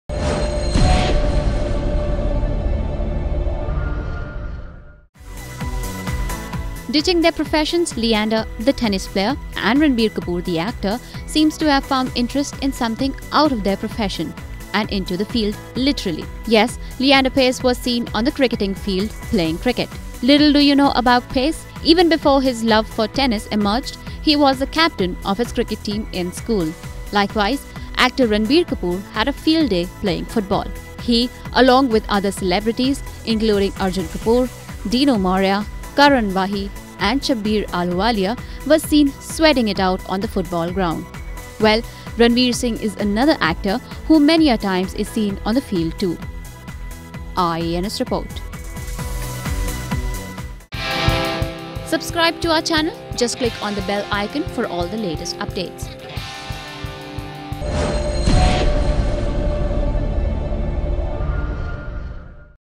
Watch Leander Paes-The Cricketer, Ranbir Kapoor-The Footballer
Ditching their professions, Leander Paes- the pro Tennis player and Ranbir Kapoor- the actor, seems to have found interest in something out of their profession and into the field, literally. Well, Ranveer Singh is another actor who many-a-times is seen on the field too.